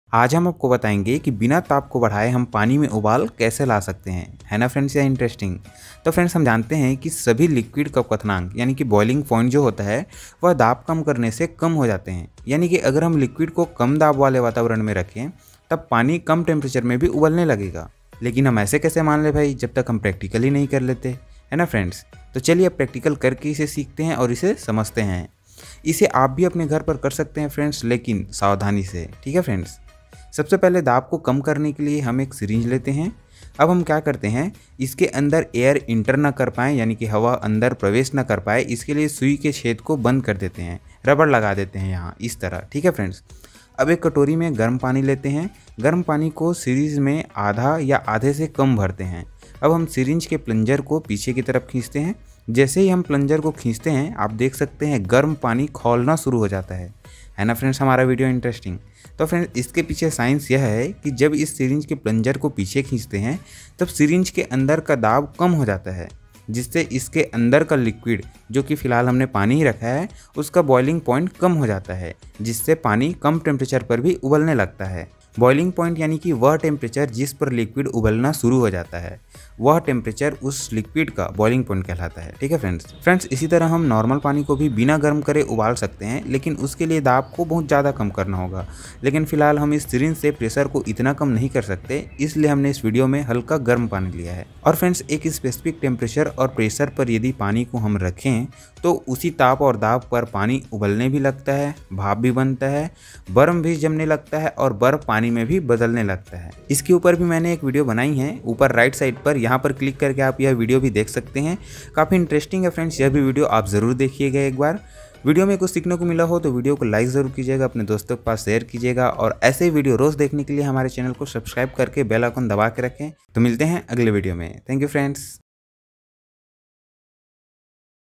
Boiling Water in Syringe || Simple Science School Experiments to do at Home
Hello Frnds ,
                         Is video me bataya gaya hai ki kaise ham room temperature par bina temp. increase kiye paani ubal sakte hai . Sath hi triple point of water ke bare me v bataya gaya hai. |